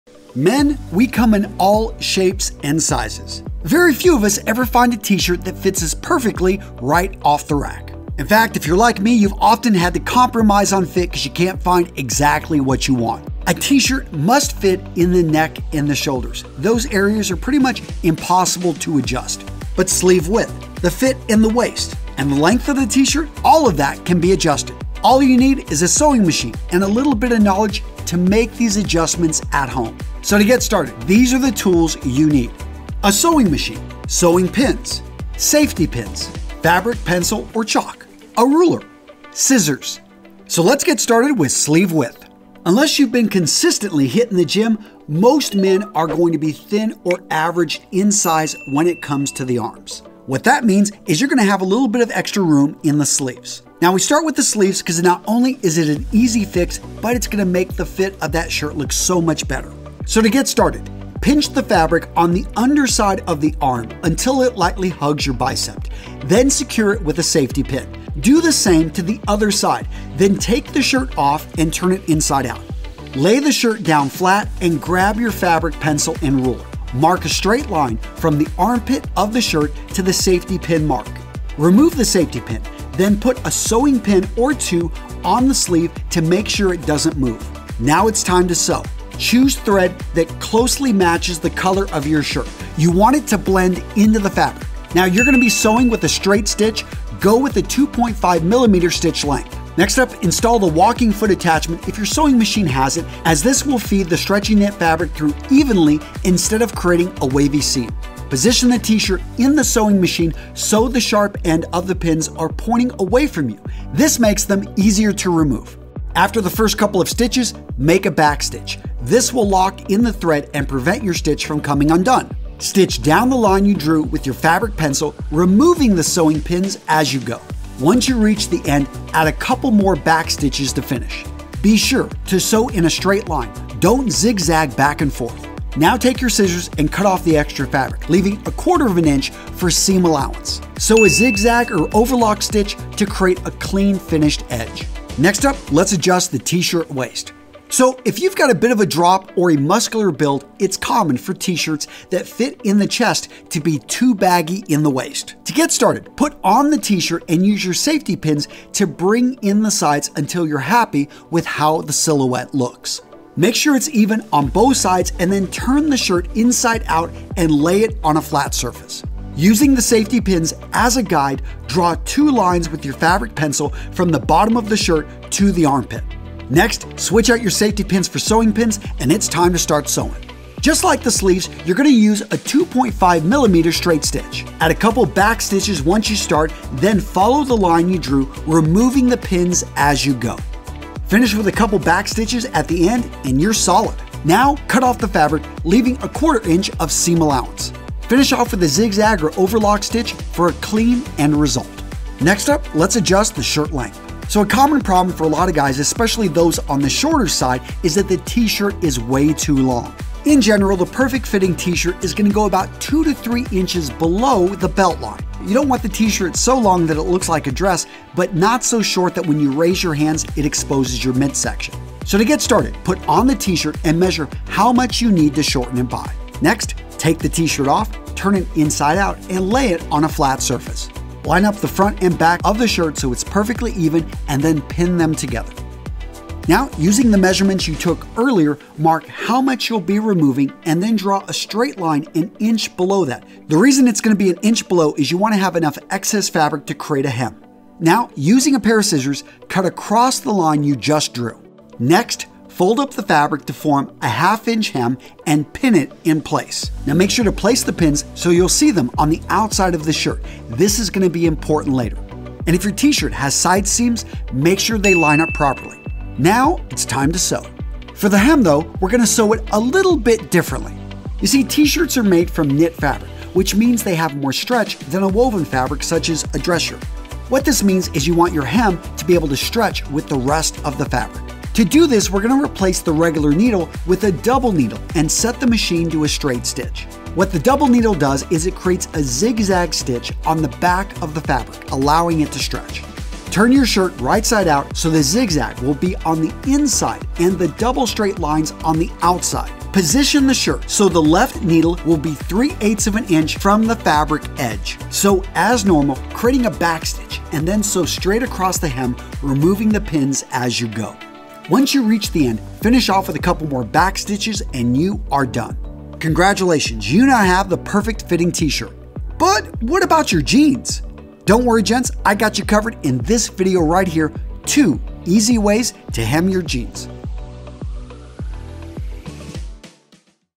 How To Tailor A T-Shirt
In today's video, I give you the steps to alter your t-shirt at home and fit your body perfectly.

📫 Shipping Address

Antonio Centeno
USA

PRODUCTS

RECOMMENDED PRODUCTS